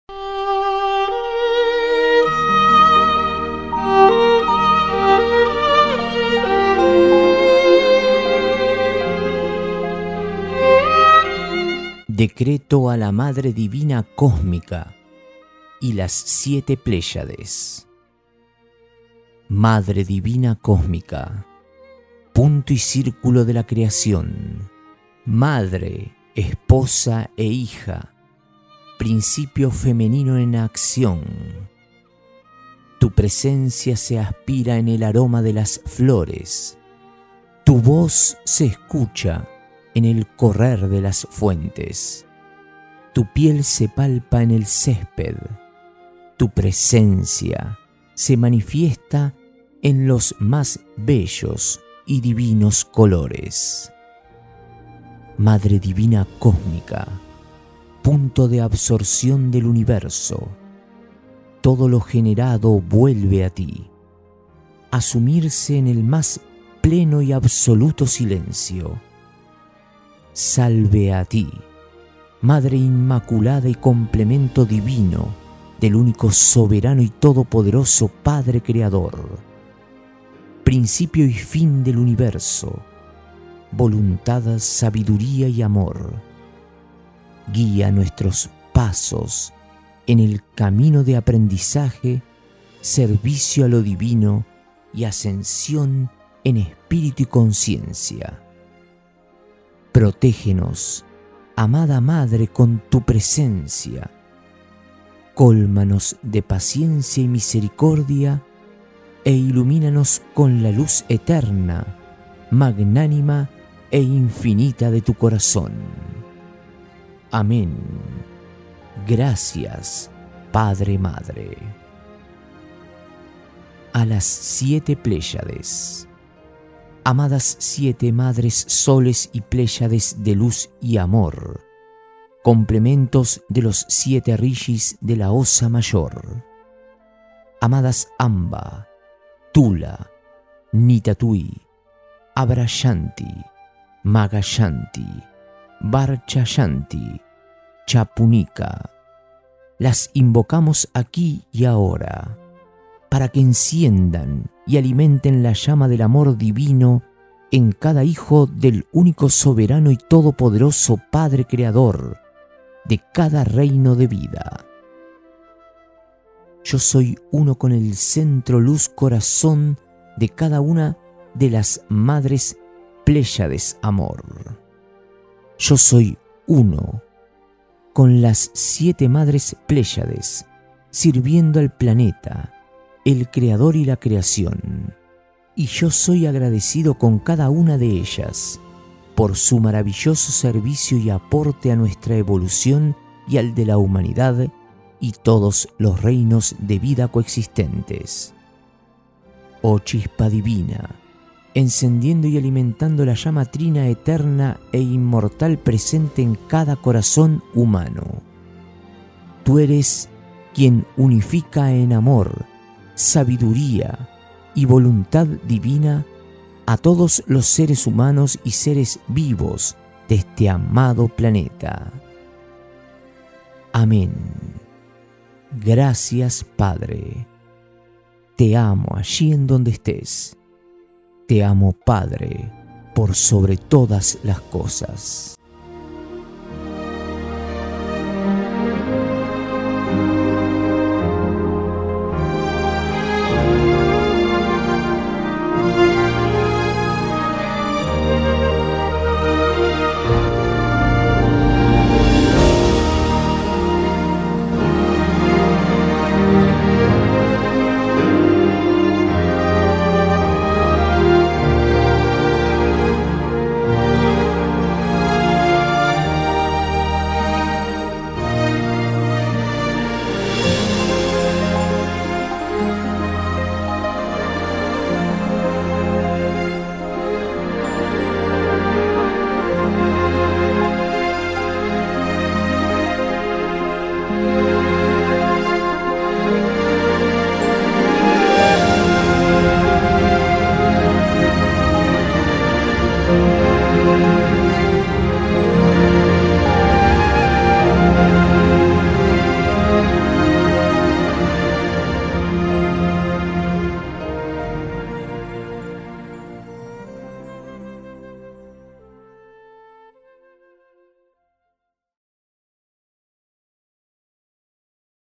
DECRETO A LA MADRE DIVINA COSMICA Y A LAS SIETE PLEYADES
DECRETO A LA MADRE DIVINA COSMICA Y LAS 7 PLEYADES

MADRE DIVINA COSMICA, PUNTO Y CIRCULO DE LA CREACIÓN, MADRE, ESPOSA E HIJA, PRINCIPIO FEMENINO EN ACCIÓN. TU PRESENCIA SE ASPIRA EN EL AROMA DE LAS FLORES, TU VOZ SE ESCUCHA EN EL CORRER DE LAS FUENTES, TU PIEL SE PALPA EN EL CÉSPED, TU PRESENCIA SE MANIFIESTA EN LOS MÁS BELLOS Y DIVINOS COLORES.

MADRE DIVINA COSMICA, PUNTO DE ABSORCIÓN DEL UNIVERSO, TODO LO GENERADO VUELVE A TI, A SUMIRSE EN EL MÁS PLENO Y ABSOLUTO SILENCIO. SALVE A TI, MADRE INMACULADA Y COMPLEMENTO DIVINO DEL UNICO SOBERANO Y TODOPODEROSO PADRE CREADOR, PRINCIPIO Y FIN DEL UNIVERSO, VOLUNTAD, SABIDURÍA Y AMOR, GUÍA NUESTROS PASOS EN EL CAMINO DE APRENDIZAJE, SERVICIO A LO DIVINO Y ASCENSIÓN EN ESPÍRITU Y CONCIENCIA. PROTEGENOS AMADA MADRE CON TU PRESENCIA, COLMANOS DE PACIENCIA Y MISERICORDIA E ILUMINANOS CON LA LUZ ETERNA, MAGNÁNIMA E INFINITA DE TU CORAZÓN.

AMÉN, GRACIAS PADRE-MADRE.

A LAS SIETE PLEYADES

AMADAS SIETE MADRES SOLES Y PLEYADES DE LUZ Y AMOR, COMPLEMENTOS DE LOS SIETE RISHIS DE LA OSA MAYOR. AMADAS AMBA, TULA, NITATUI, ABRASHANTI, MAGASHANTI, BARCHASHANTI, CHAPUNICA, LAS INVOCAMOS AQUÍ Y AHORA, PARA QUE ENCIENDAN Y ALIMENTEN LA LLAMA DEL AMOR DIVINO EN CADA HIJO DEL ÚNICO SOBERANO Y TODOPODEROSO PADRE CREADOR, DE CADA REINO DE VIDA.

YO SOY UNO CON EL CENTRO LUZ CORAZÓN DE CADA UNA DE LAS MADRES PLEYADES AMOR. YO SOY UNO CON LAS SIETE MADRES PLEYADES, SIRVIENDO AL PLANETA, EL CREADOR Y LA CREACIÓN Y YO SOY AGRADECIDO CON CADA UNA DE ELLAS, POR SU MARAVILLOSO SERVICIO Y APORTE A NUESTRA EVOLUCIÓN Y AL DE LA HUMANIDAD Y TODOS LOS REINOS DE VIDA COEXISTENTES.

OH CHISPA DIVINA, ENCENDIENDO Y ALIMENTANDO LA LLAMA TRINA, ETERNA E INMORTAL PRESENTE EN CADA CORAZÓN HUMANO, TU ERES QUIEN UNIFICA EN AMOR, SABIDURÍA Y VOLUNTAD DIVINA, A TODOS LOS SERES HUMANOS Y SERES VIVOS DE ESTE AMADO PLANETA.

AMEN.

GRACIAS PADRE.

TE AMO ALLÍ EN DONDE ESTÉS. TE AMO PADRE POR SOBRE TODAS LAS COSAS.

ERIC